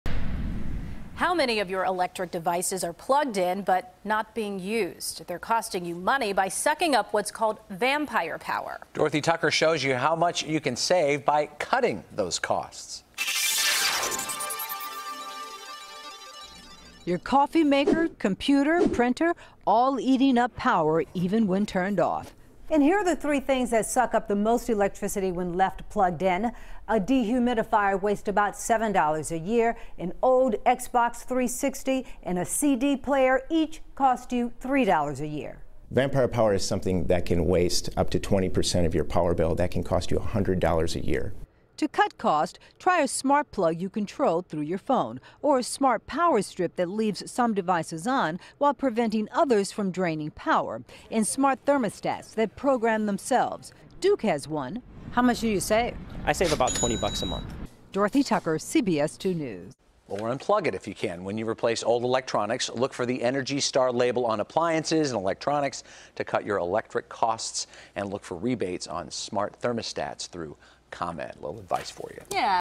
How To Vanquish Electricity Vampires
CBS 2's Cost Cutter Dorothy Tucker reports.